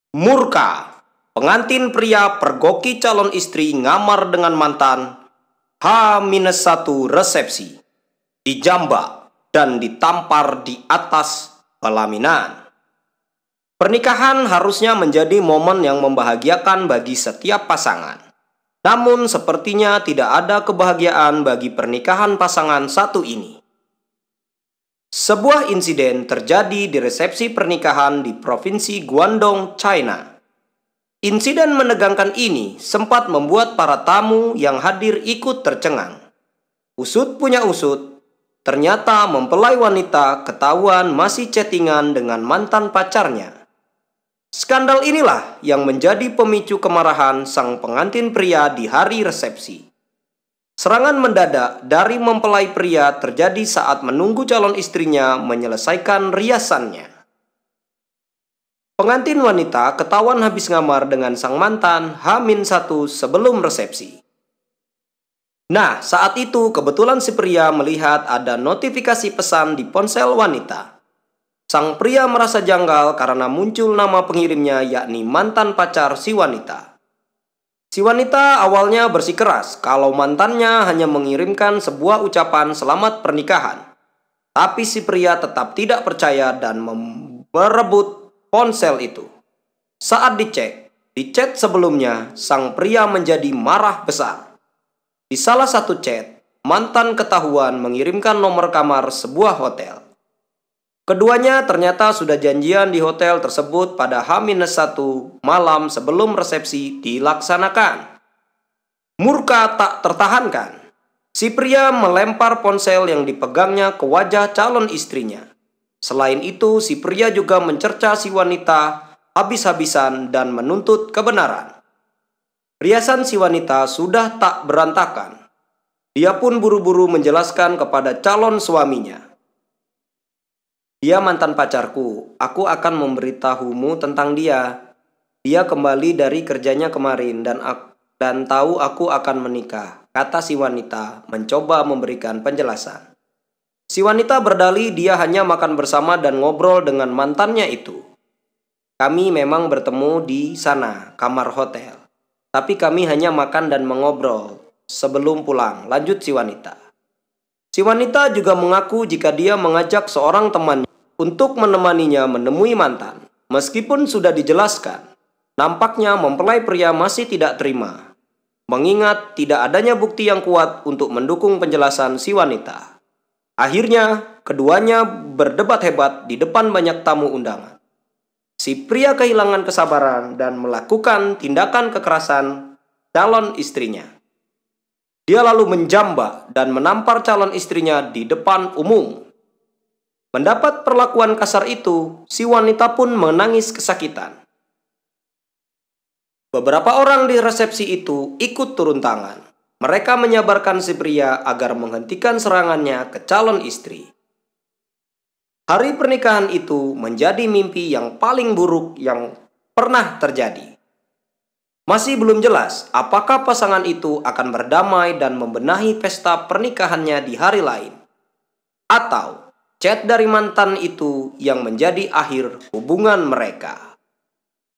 MURKA! Pengantin Pria Pergoki Calon Istri Ngamar dengan Mantan H-1 Resepsi
MURKA! Pengantin Pria Pergoki Calon Istri Ngamar dengan Mantan H-1 Resepsi, Dijambak dan Ditampar di Atas Pelaminan.

Pernikahan harusnya menjadi momen yang membahagiakan bagi setiap pasangan. Namun, sepertinya tidak ada kebahagiaan bagi pernikahan pasangan satu ini. Sebuah insiden terjadi di resepsi pernikahan di Provinsi Guandong, China.

Insiden menegangkan ini sampai membuat para tamu yang hadir ikut tercengang. Usut punya usut, ternyata mempelai wanita ketahuan masih chatingan dengan mantan pacarnya. Skandal inilah yang menjadi pemicu kemarahan sang pengantin pria di hari resepsi. Serangan mendadak dari mempelai pria terjadi saat menunggu calon istrinya menyelesaikan riasanya.

Pengantin wanita ketahuan habis ngamar dengan sang mantan H-1 sebelum resepsi. Nah, saat itu, kebetulan si pria melihat ada notifikasi pesan di ponsel wanita. Sang pria merasa janggal karena muncul nama pengirimnya yakni mantan pacar si wanita.

Si wanita awalnya bersikeras kalau mantannya hanya mengirimkan sebuah ucapan selamat pernikahan. Tapi si pria tetap tidak percaya dan merebut ponsel itu. Saat dicek di chat-chat sebelumnya, sang pria menjadi marah besar. Di salah satu chat, mantan ketahuan mengirimkan nomor kamar sebuah hotel.

Keduanya ternyata sudah janjian di hotel tersebut pada H-1 malam sebelum resepsi dilaksanakan. Murka tak tertahankan, si pria melempar ponsel yang dipegangnya ke wajah calon istrinya. Selain itu, si pria juga mencerca si wanita habis-habisan dan menuntut kebenaran. Riasan si wanita sudah tak berantakan, diapun buru-buru menjelaskan kepada calon suaminya.

"Dia mantan pacarku, aku kan memberitahumu tentang dia, Dia kembali dari kerjanya kemarin dan tahu aku akan menikah," kata si wanita mencoba memberikan penjelasan.

Si wanita berdalih dia hanya makan bersama dan ngobrol dengan mantannya itu.

"Kami memang bertemu di sana (kamar hotel), tapi kami hanya makan dan mengobrol sebelum pulang," lanjut si wanita.

Si wanita juga mengaku jika dia mengajak seorang temannya untuk menemaninya menemui mantan. Meskipun sudah dijelaskan, nampaknya mempelai pria masih tidak terima. Mengingat tidak adanya bukti yang kuat untuk mendukung penjelasan si wanita